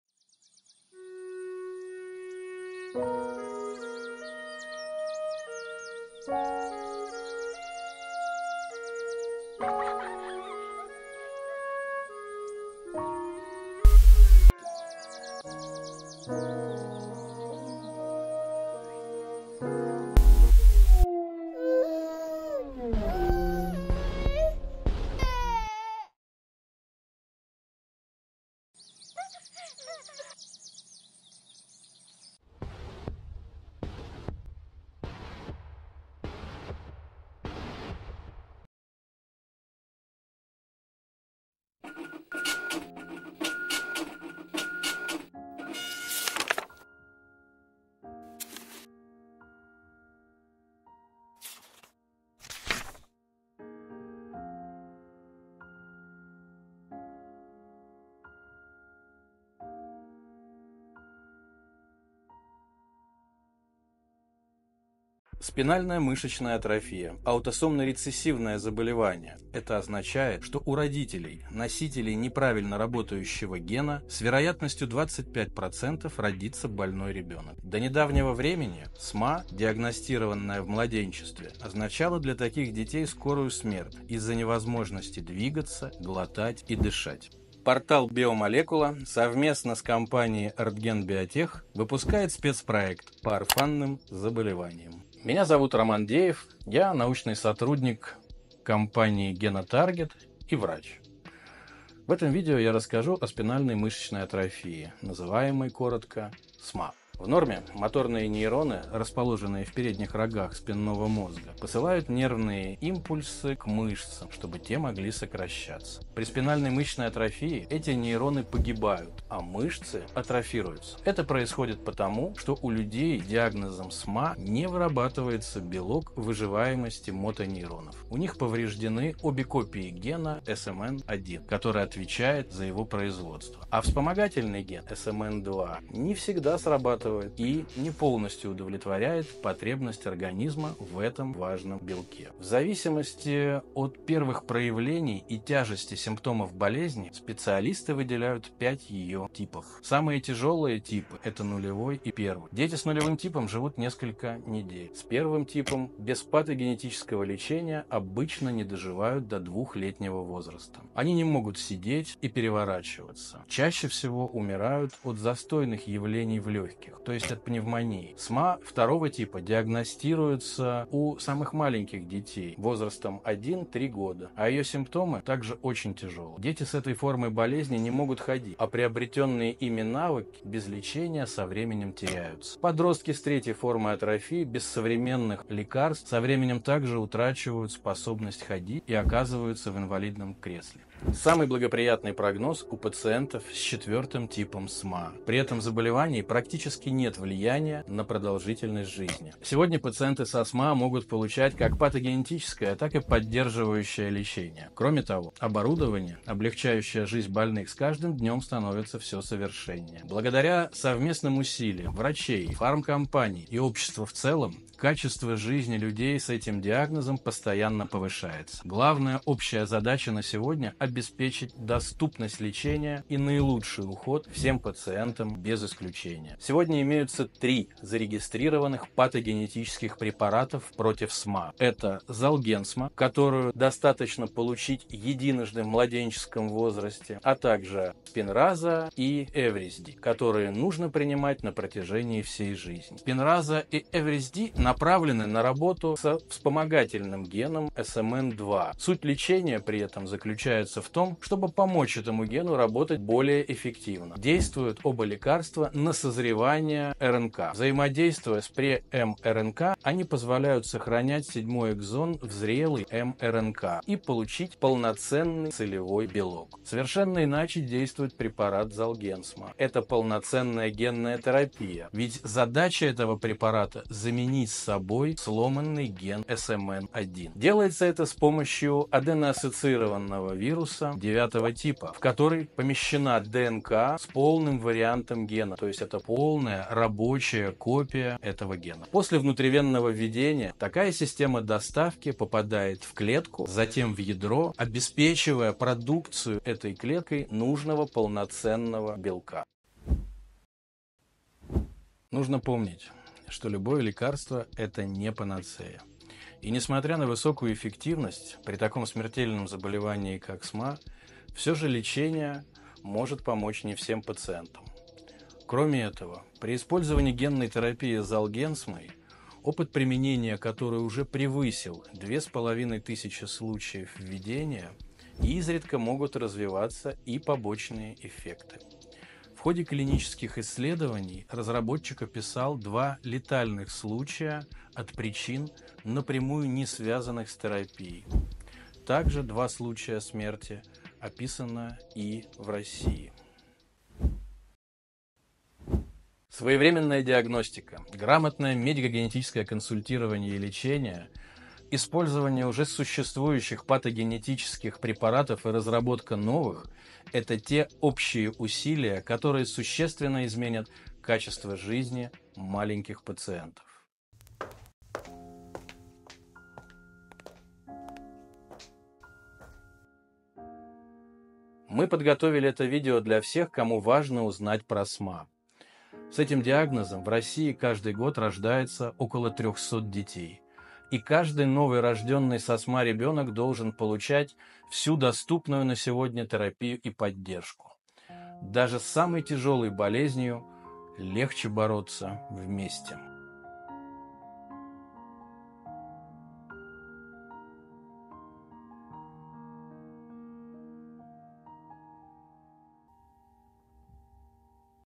29 февраля. День редких болезней. Спинальная мышечная атрофия
Этим видео Биомолекула совместно с компанией «Артген биотех» продолжает рассказ про редкие нейромышечные заболевания, приуроченный ко Дню орфанных болезней, который мы отмечаем сейчас, 29 февраля. В ролике мы расскажем про спинальную мышечную атрофию (СМА): в чем причина болезни, какие выделяют типы СМА, что за препараты есть на сегодняшний день и насколько безопасна генная терапия некогда самым дорогим лекарством на планете — Золгенсма.

📌 Подробнее прочитать про спинальную мышечную атрофию можно в статье «СМА: три буквы — три лекарства»

📌 Наш спецпроект по орфанным заболеваниям

Это позволит расширить круг осведомленных о спинальной мышечной атрофии, а СМАйлики — так ласково называют больных — смогут почувствовать нашу поддержку!